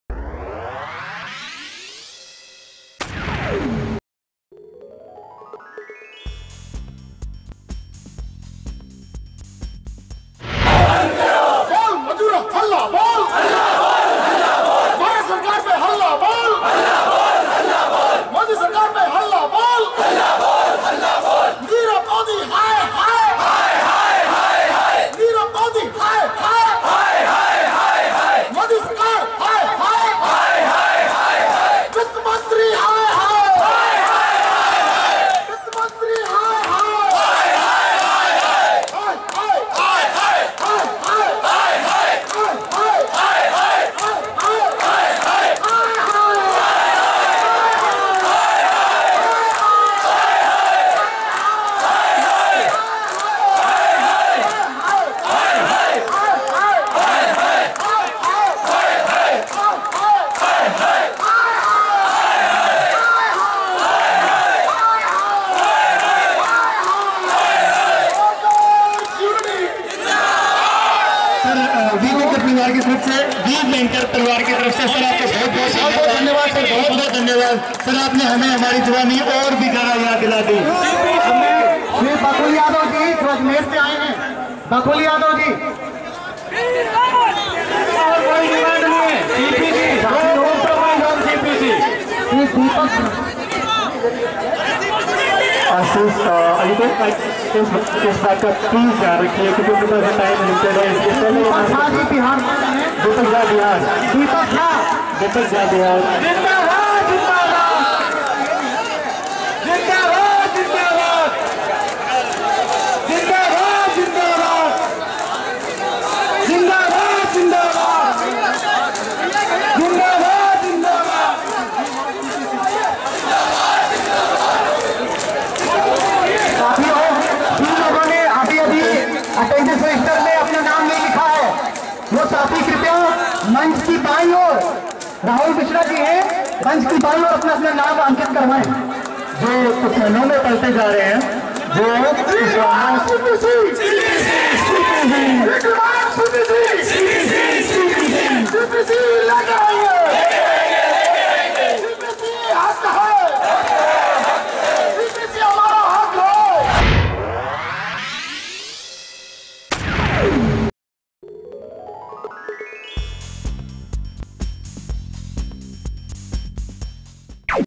Modi Sarkar से हो रही है लोगो को परेशानी-We Bankers protest for CPC against BJP-SM News
Modi Sarkar से हो रही है लोगो को परेशानी